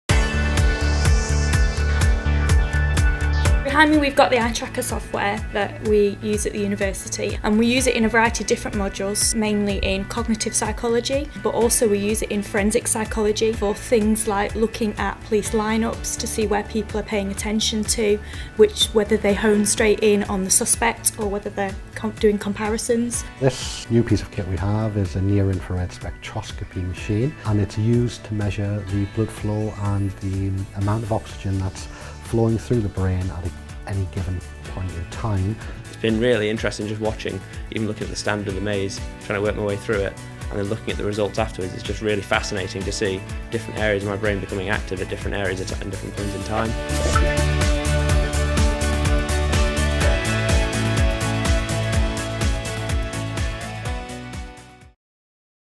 Psychology
Studying Psychology at the School of Social Sciences, Humanities & Law gives you access to outstanding facilities, including eye-trackers, EEG and Near-infrared Spectroscopy (NIRS). You also get to use specialist interviewing labs and our replica courtroom.